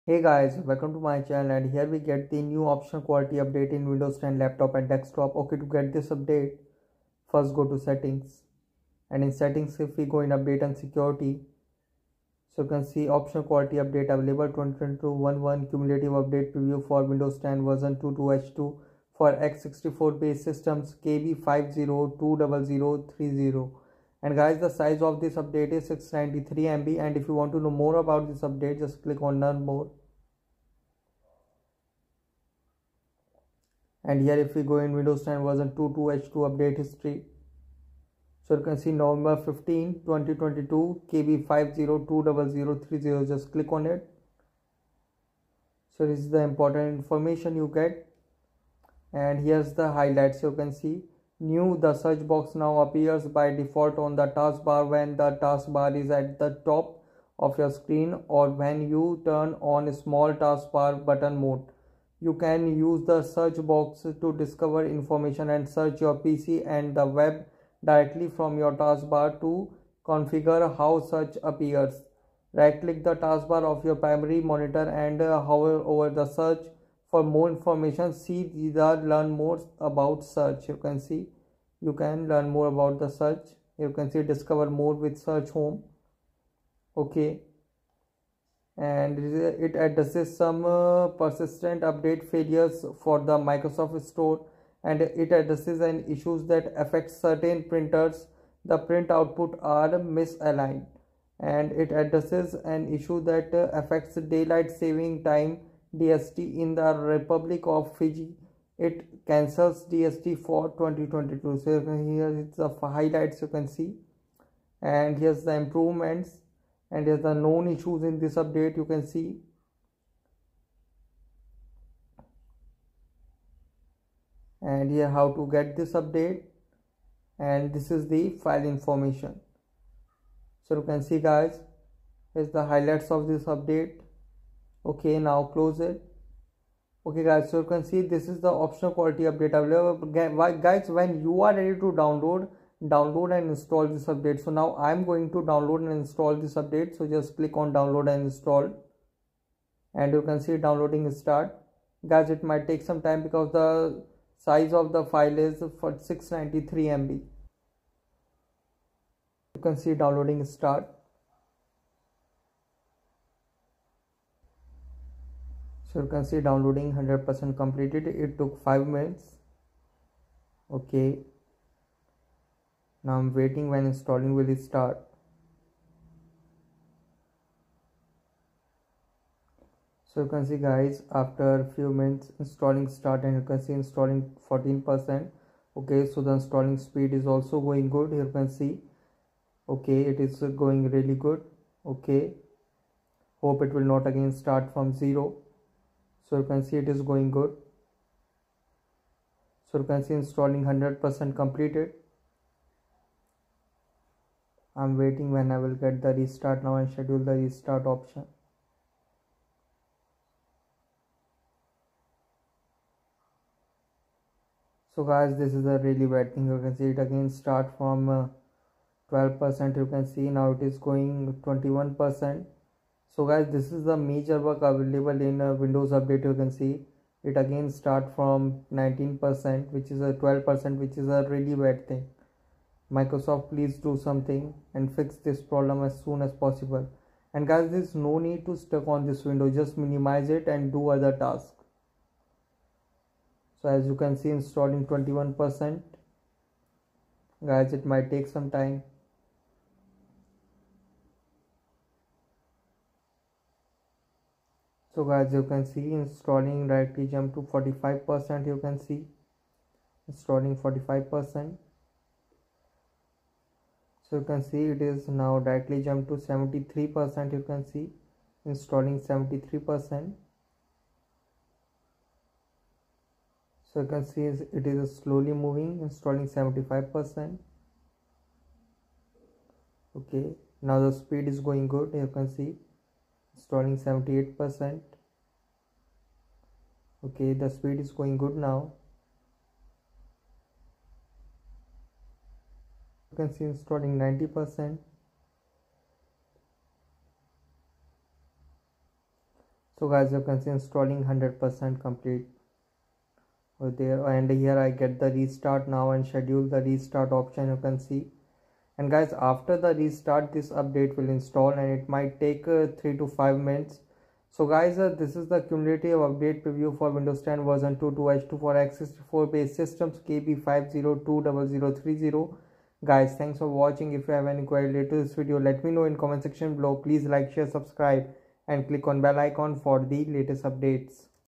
Windows 10 Cumulative Update For Version 22H2 x64 Based Systems - Again Bad Experience!
The all new cumulative update KB5020030 for Windows 10 Version 22H2 for x64-based Systems is available to download and install. This is the Windows 10 Version 22H2 second optional quality update which will improve the Windows.